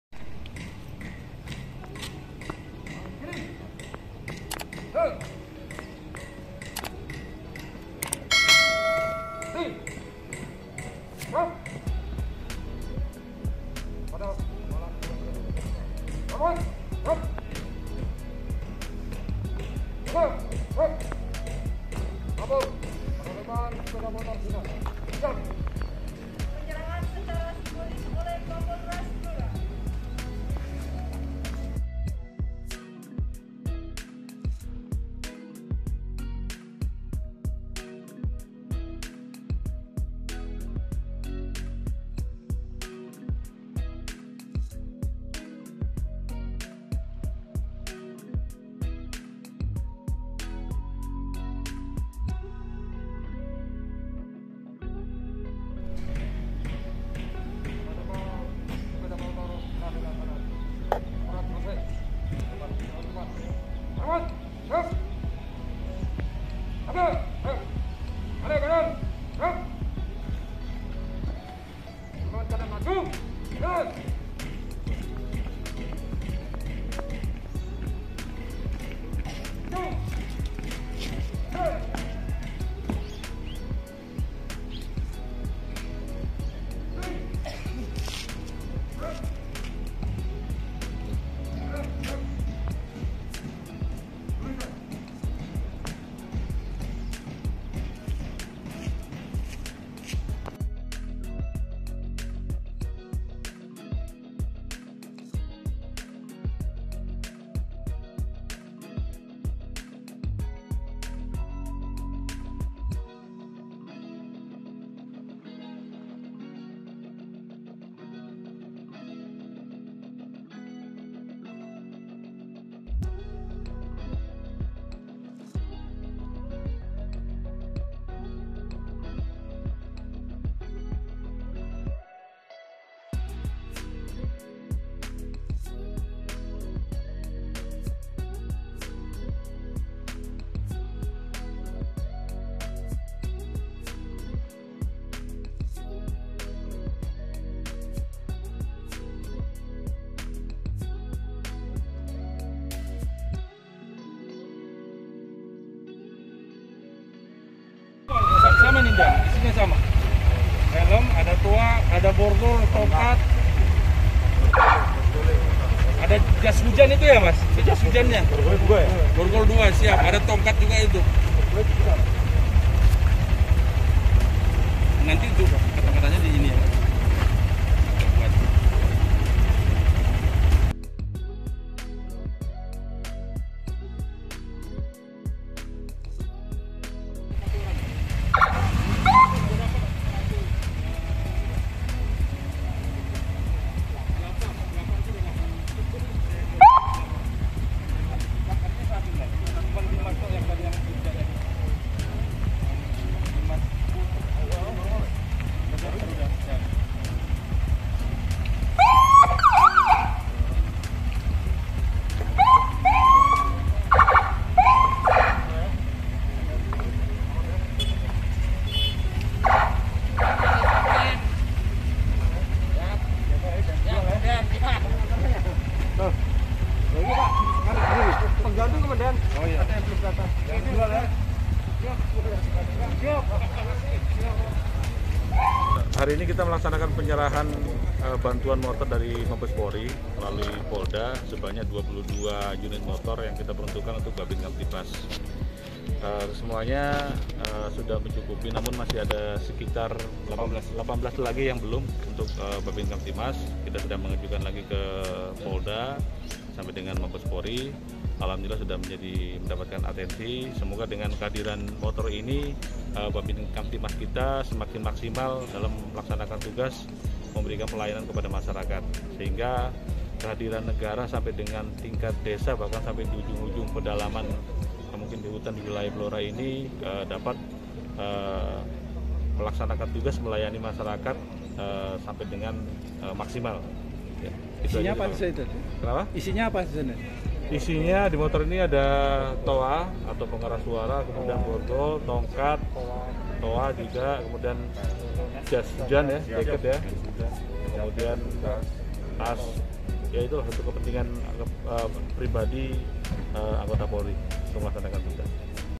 Kapolres Blora Serahkan Motor Dinas Bhabinkamtibmas
Kapolres Blora Serahkan Ranmor Dinas Bhabinkamtibmas

Kapolres Blora Polda Jawa Tengah AKBP Wiraga Dimas Tama,SIK pimpin langsung jalannya apel penyerahan kendaraan dinas Bhabinkamtibmas di halaman belakang Mapolres Blora, Rabu, (27/10/2021).

Hadir dalam kegiatan tersebut Pejabat Utama Polres Blora, Para Perwira Serta anggota, dan 22 Bhabinkamtibmas Polsek Jajaran Polres Blora penerima kendaraan motor dinas.

Dalam Apel tersebut Kapolres Blora menyerahkan Kendaran dinas roda dua secara simbolis kepada perwakilan anggota Bhabinkamtibmas. Adapun hari ini Kapolres menyerahkan 22 unit sepeda motor Merk Honda Verza

Kapolres Blora AKBP Wiraga Dimas Tama,SIK menyampaikan bahwa Bhabinkamtibmas adalah ujung tombak kepolisian di desa, bisa dikatakan Bhabinkamtibmas adalah Kapolri di desa. Sehingga tanggungjawabnya besar, untuk sarana dan prasarana yang telah diberikan, salah satunya adalah kendaraan dinas. Kapolres berpesan agar dijaga dan dirawat dengan baik, sehingga bisa menjadi penunjang dalam pelaksanaan tugas melayani masyarakat.

“Mohon dirawat dan dijaga baik baik, kendaraan dinas adalah titipan negara untuk memberikan pelayanan kepada masyarakat, dengan mendapat kendaraan dinas diharapkan Bhabinkamtibmas semakin semangat,” beber Kapolres AKBP Wiraga.

Adapun dalam kendaraan dinas sepeda motor Honda Verza tersebut juga telah disiapkan beberapa peralatan kepolisian yang ada didalam Box Motor, meliputi borgol, tongkat Polri, jas hujan, hingga Mega Phone atau alat pengeras suara yang memang disiapkan untuk melaksanakan tugas melayani masyarakat.

Kapolres manambahkan, menyikapi musim pancaroba menjelang musim penghujan. Agar anggota Bhabinkamtibmas aktif mengajak warga untuk waspada terhadap bencana alam.
"Tingkatkan sambang ke wilayah binaan, terutama kawasan yang rawan bencana alam. Ingatkan warganya agar selalu waspada terhadap bencana alam," pungkas Kapolres Blora.